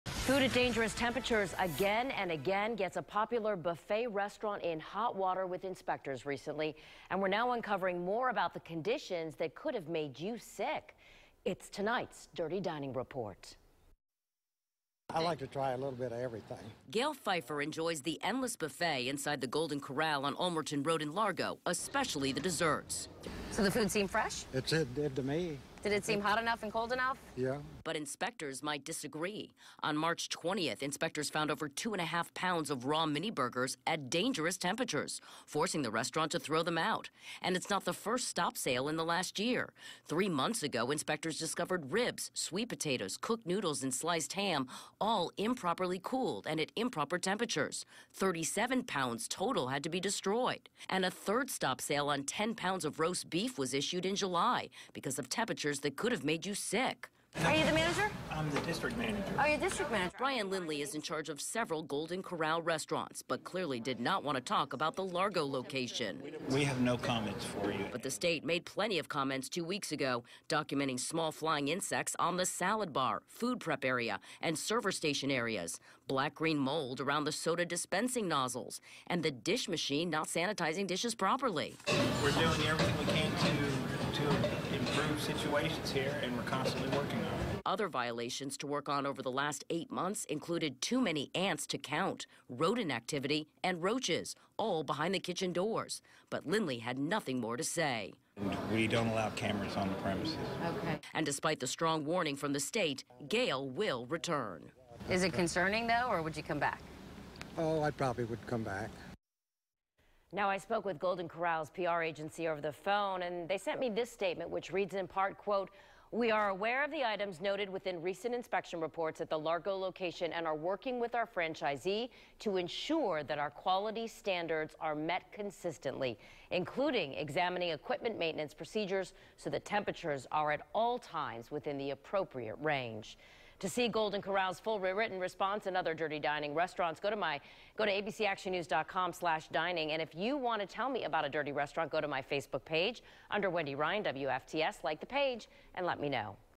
Dirty Dining: Golden Corral
Golden Corral is known for the large variety of food on its buffet but inspectors found a large variety of food at dangerous temperatures, issuing a strong warning for the popular restaurant.